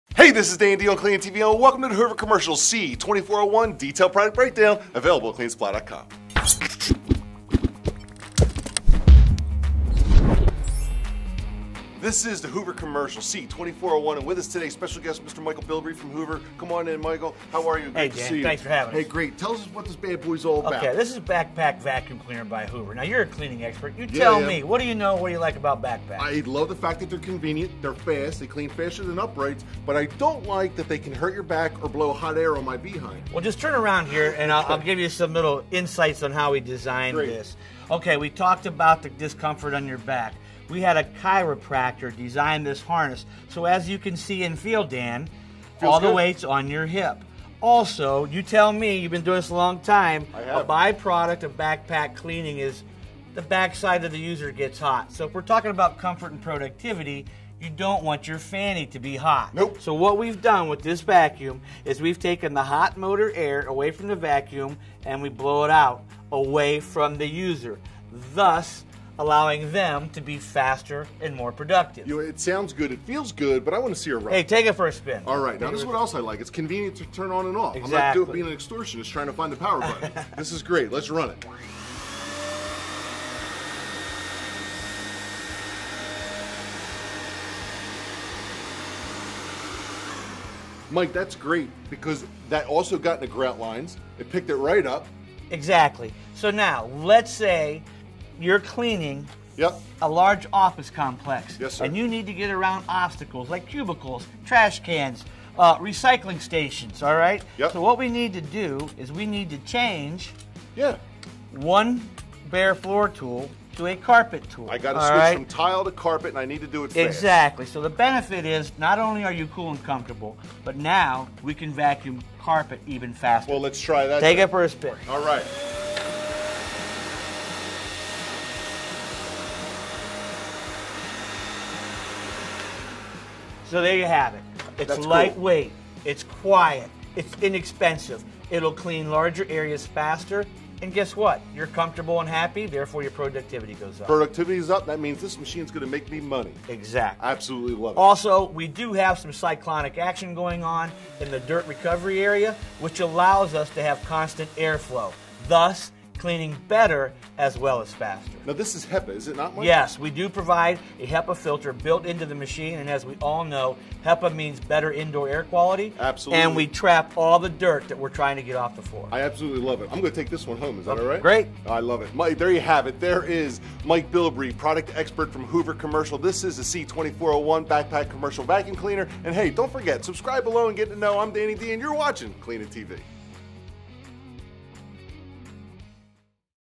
Hoover C2401 Bagless Backpack Vacuum Cleaner, Black
Hoover Backpack Bagless Vacuum

Hoover C2401 Backpack Vacuum
Lightweight and easy to use, this backback vacuum weighs only nine pounds. The deluxe nylon woven shoulder harness allows for for comfortable, trouble-free use. Chiropractor designed, ergonomic harness includes sternum clip to secure shoulder straps. The clear dome lid provides a clear view of debris. The extra long 48' 3-wire quick change cord allows the user to clean large areas, switching outlets less often. Additional features include: 5' positive locking hose, 57' two-piece two-bend aluminum wand, 2.5" dusting brush, 5" upholstery tool and adapter, 17" crevice tool, 14" floor tool and 14" all-purpose floor tool. The Hoover Backpack Vacuum uses either a shake-out cloth inner filter bag or disposable debris bag. The Hypercone filter made with HEPA media allows for consistent air flow without loss of suction. Current: 8.5 A; Tank Capacity: 6.4 qt; Color(s): Black; Weight: 9.2 lbs.

Hoover Commercial Bagless Backpack Vacuum Cleaner, Black, Sold as Each